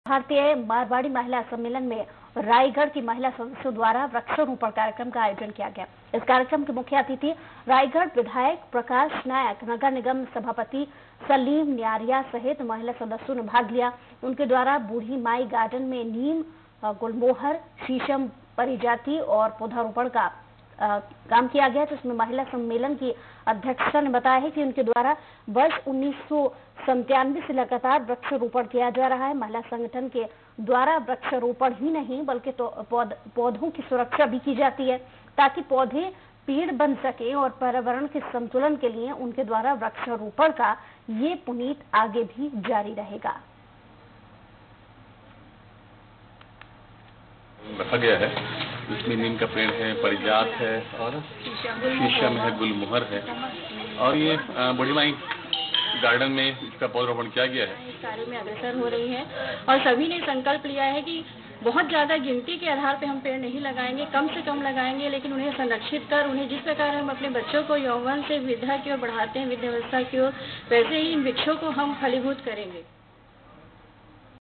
अखिल भारतीय मारवाड़ी महिला सम्मेलन की सदस्यों ने किया वृक्षारोपण कार्यक्रम का आयोजन
अखिल भारतीय मारवाड़ी महिला सम्मेलन की सदस्यों ने किया वृक्षारोपण कार्यक्रम का आयोजन, लोगों से की पेड़ लगाने की अपील